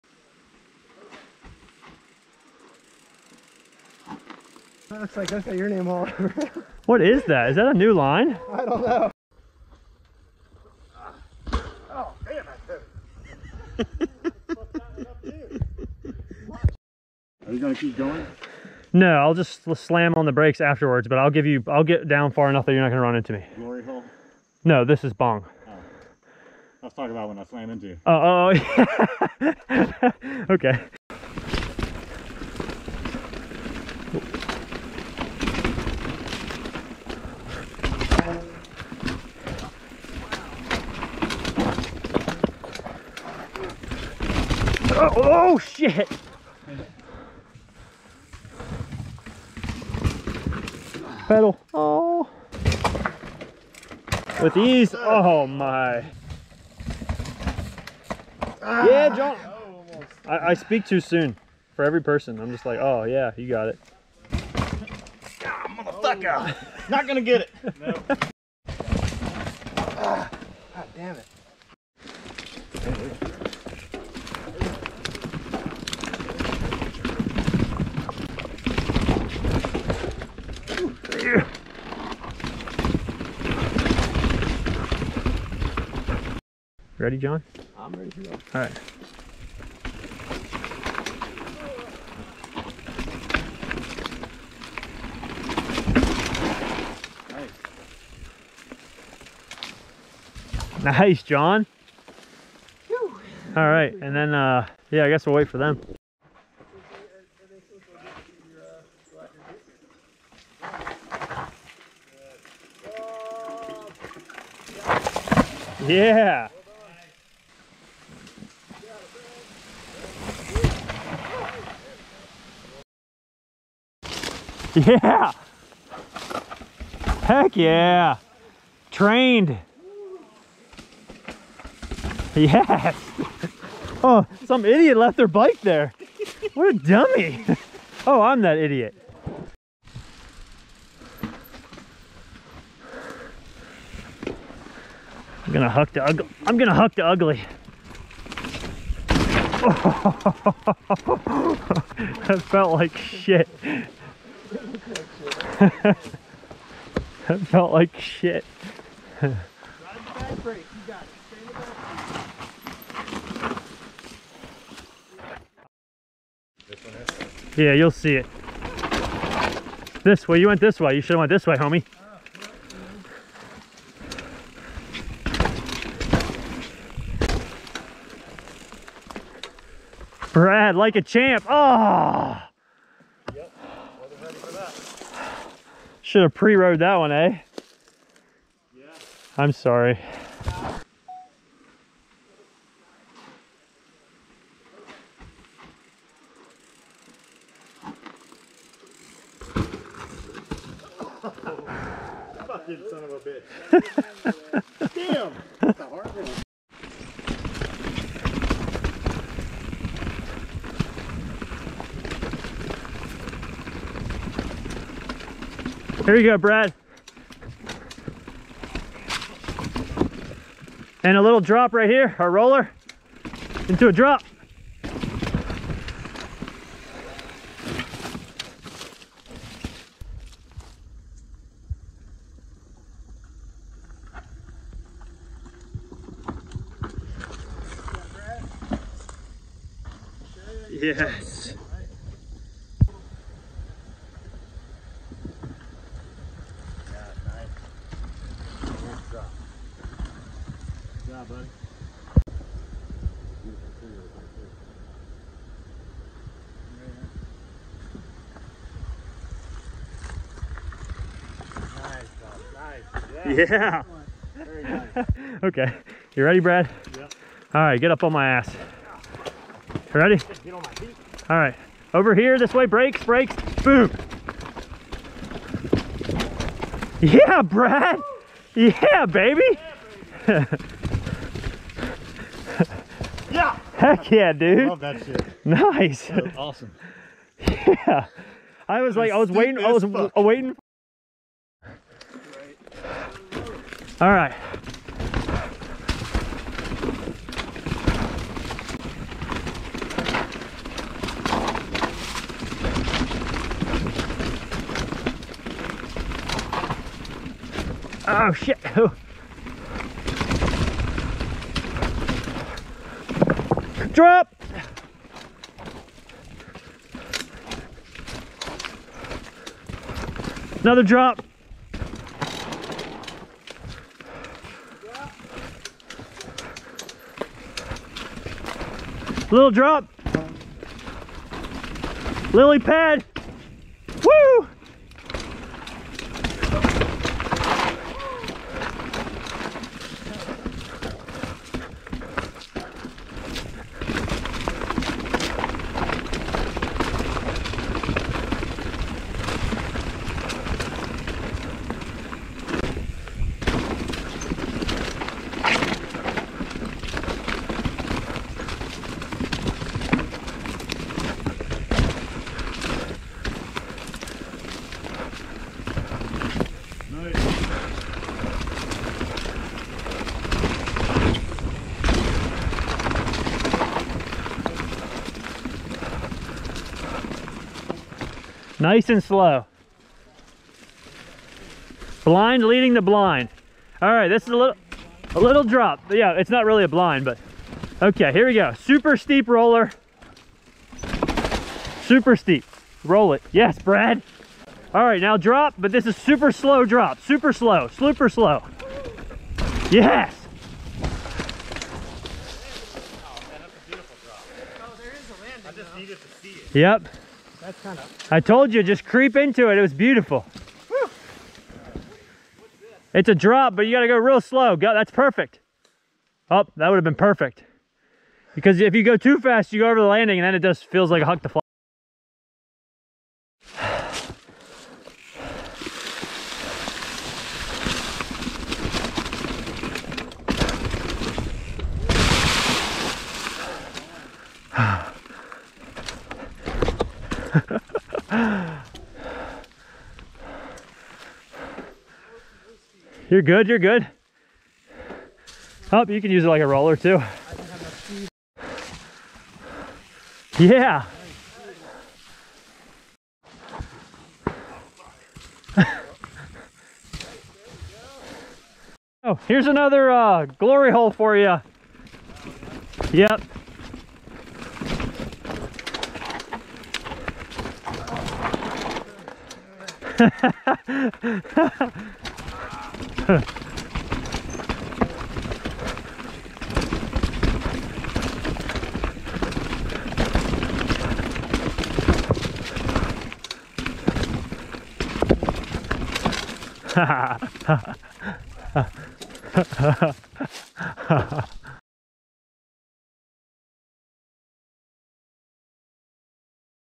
Northern Michaux with a GROUP
After that previous video of scoping out all the trails I wanted to ride in Northern Michaux: I invited Jon and @Brad_AF  , and we met up with @MichauxGravityRiders a lot of these are the same trails from the previous video, but with lots of 3rd person so you can actually see the size of the features.